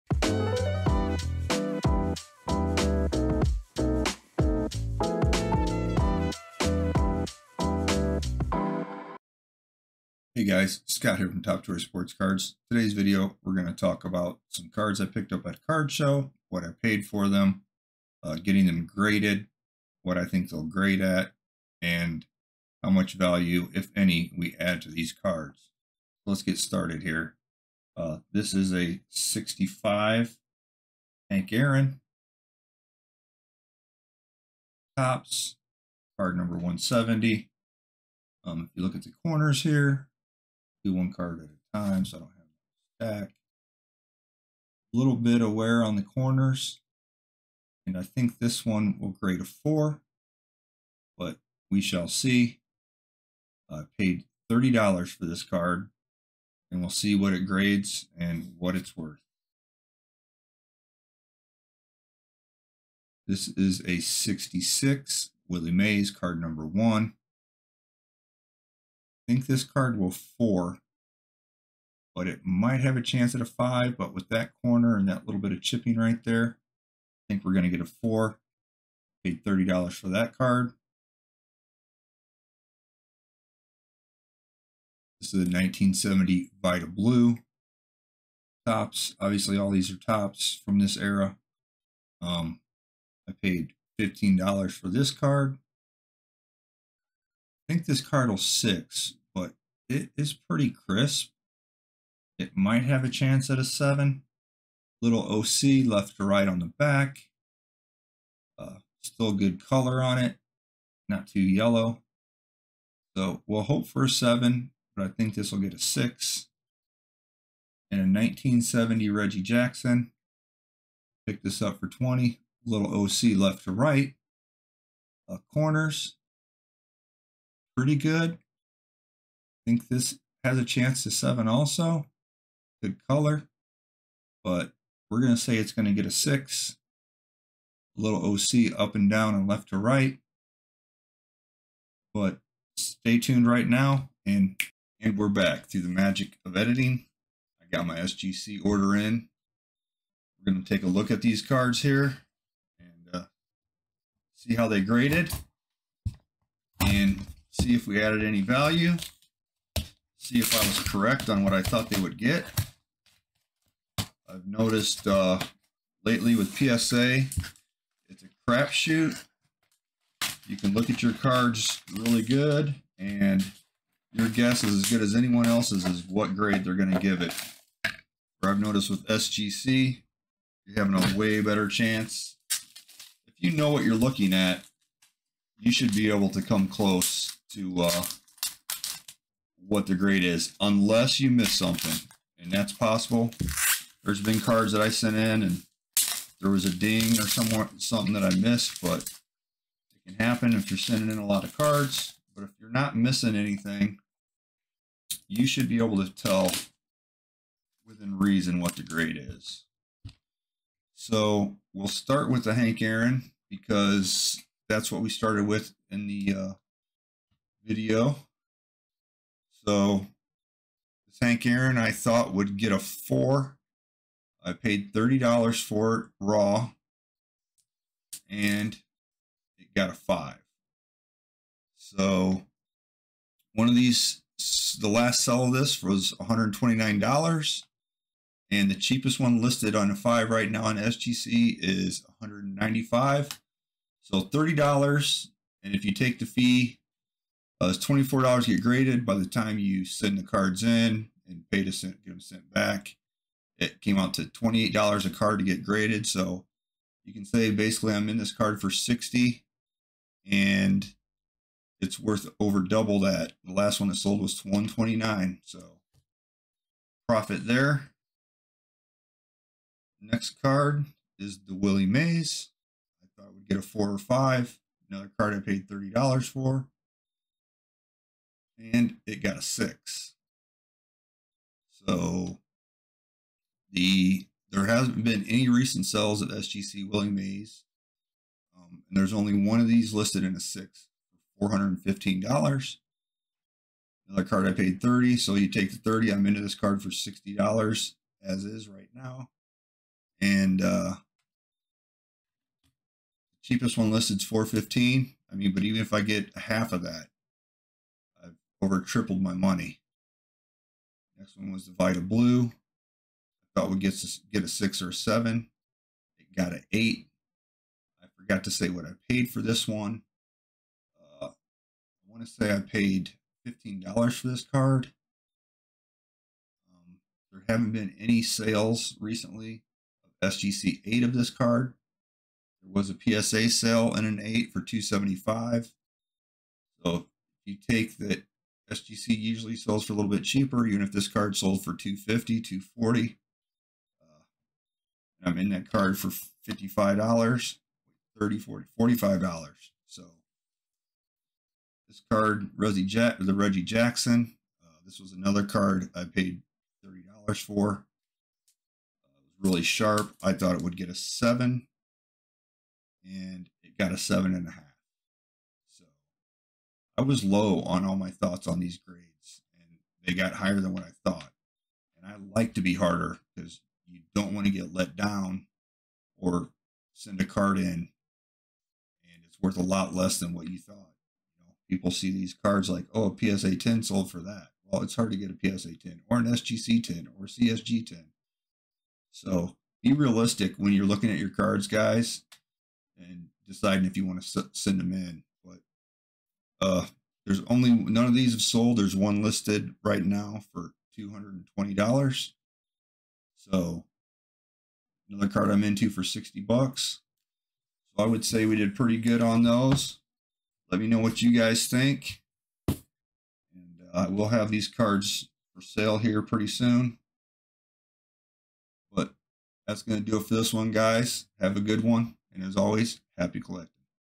Did I Add Value To These Vintage Cards Grading With SGC?!!!
In this video I go over 4 vintage cards that I submitted to SGC, What I think they will grade and what i paid and then reveal what they received from SGC and what they might be worth now.